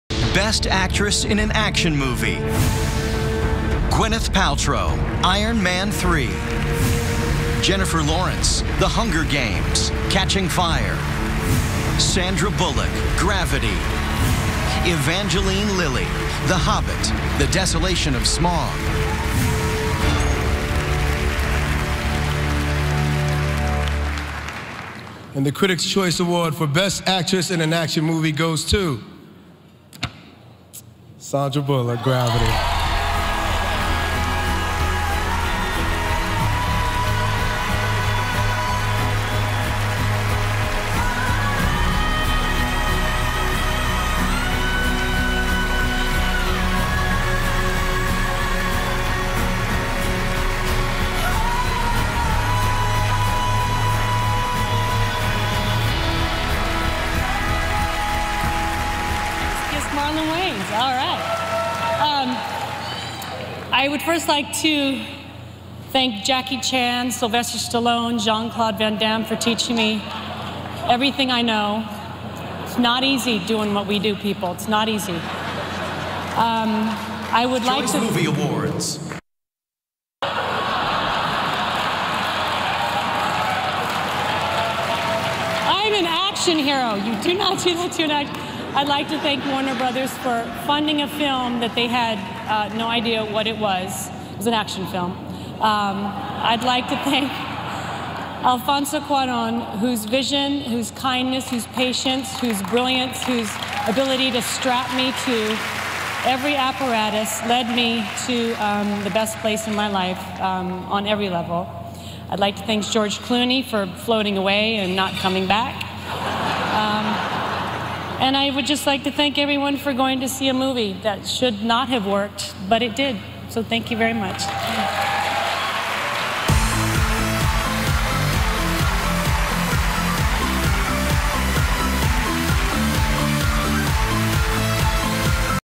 Sandra Bullock won Best Actress in An Action Movie
Sandra Bullock won Best Actress in An Action Movie at 19th Critics Choice Movie Awards.